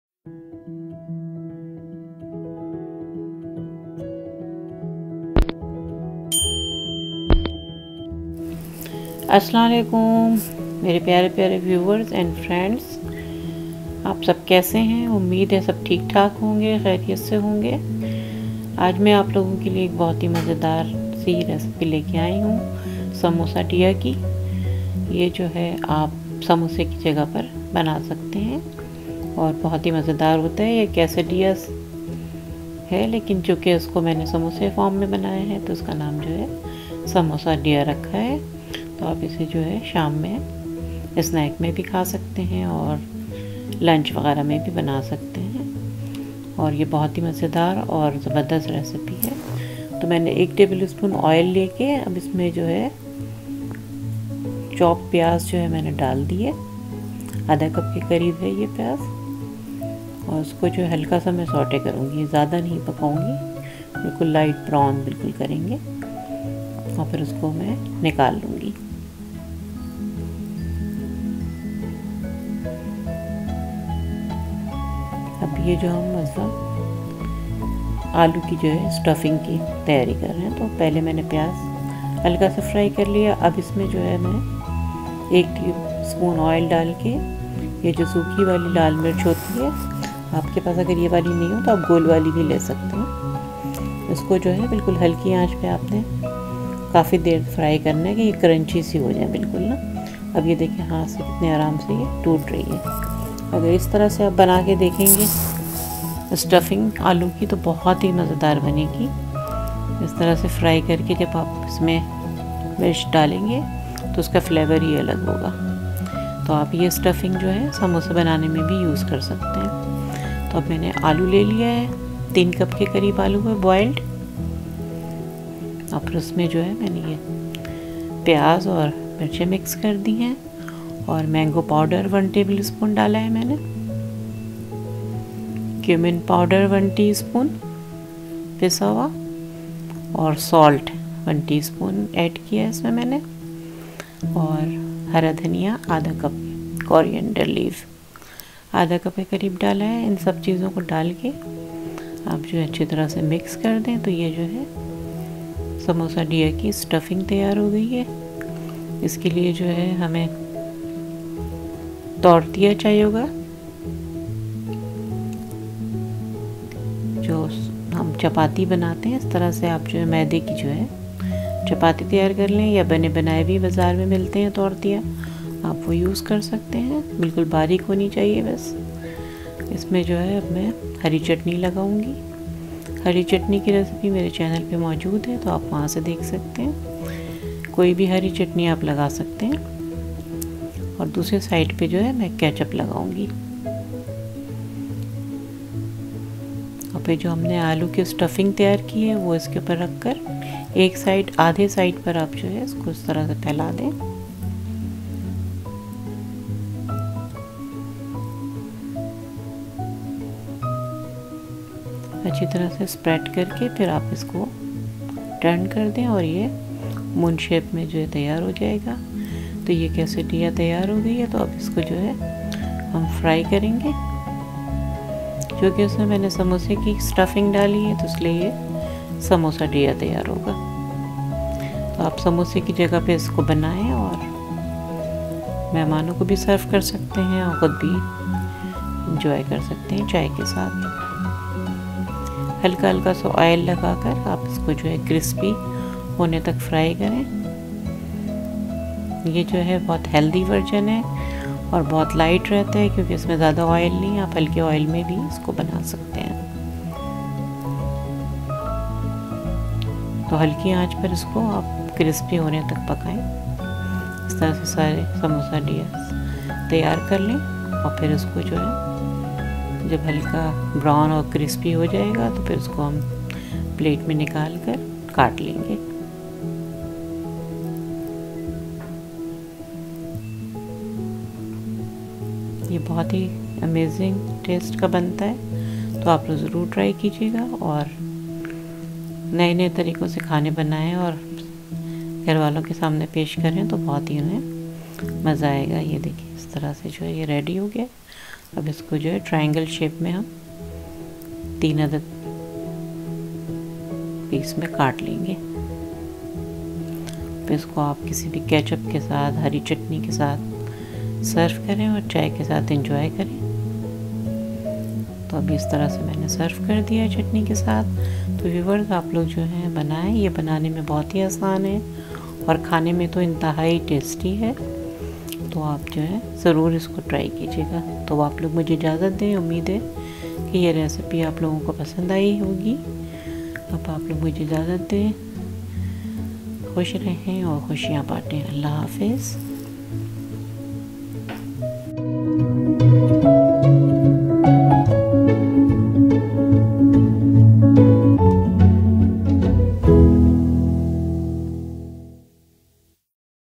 Samosadilla Recipe | Potato Quesadilla | Vegi Quesadilla | Urdu Hindi
A very delicious and tasty Samosadilla recipe. Recipe in Urdu Hindi.

INGREDIENTS

Boiled potatoes - 1/2 kg
Mango powder - 1 tbsp
Green coriander leaves - 1/2 cup
Tortillas - 8
Green chutney as required
Ketchup as required